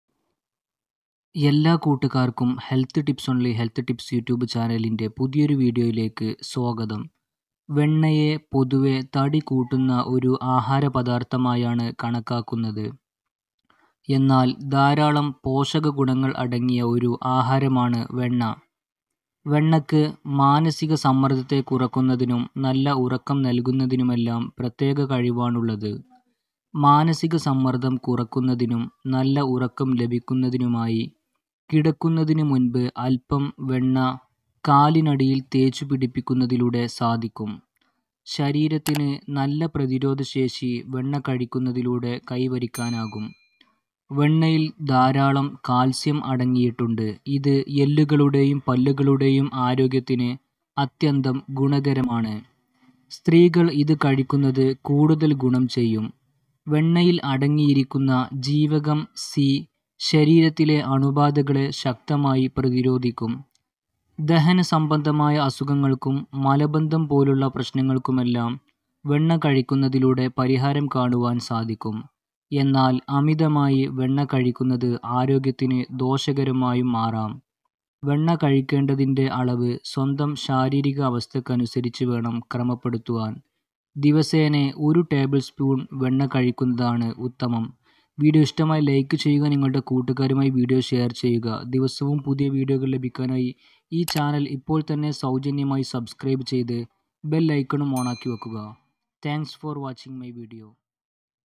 Are you aware of these qualities of Butter? | Health Tips Only Health Tips
വെണ്ണയെ പൊതുവെ തടി കൂട്ടുന്ന ഒരു ആഹാര പദാർത്ഥമായാണ് കണാക്കാക്കുന്നത്. എന്നാൽ ധാരാളം പോഷക ഗുണങ്ങൾ അടങ്ങിയ ഒരു ആഹാരമാണ് വെണ്ണ. വെണ്ണക്ക് മാനസിക സമ്മർദ്ദത്തെ കുറക്കുന്നതിനും നല്ല ഉറക്കം നൽകുന്നതിനുമെല്ലാം പ്രത്യേക കഴിവാണുള്ളത്.